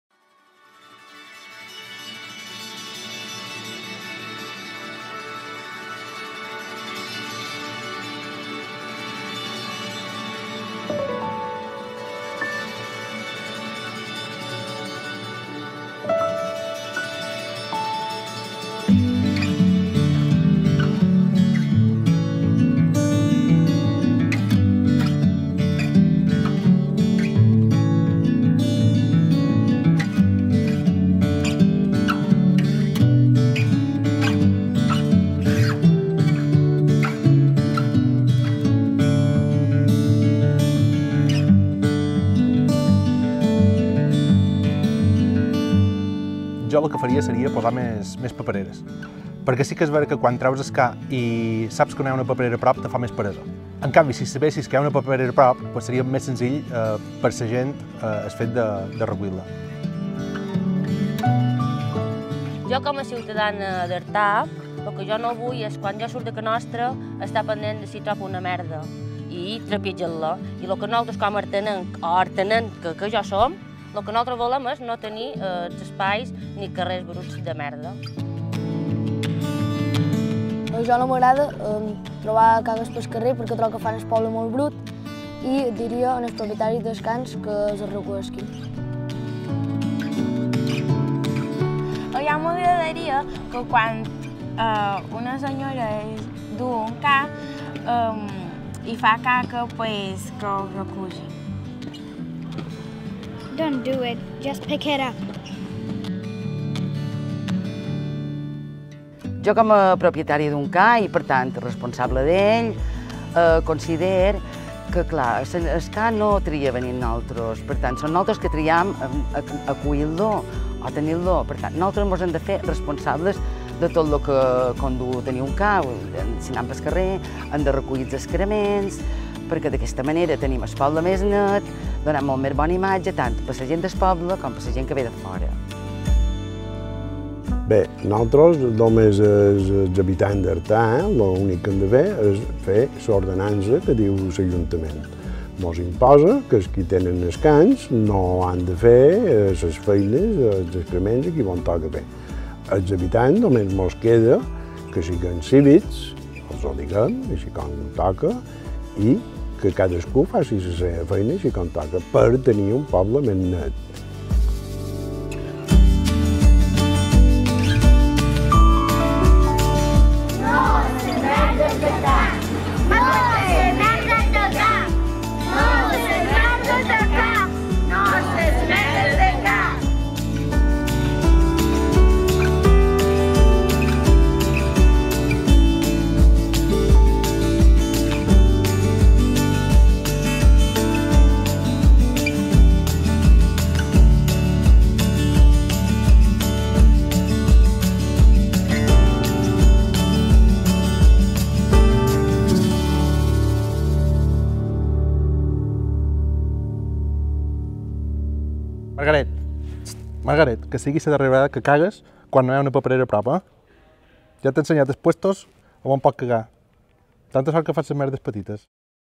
Artà. Passejar pel poble no ha de ser questio de sort.
Vídeo de l'Ajuntament d'Artà sobre la campanya de civisme.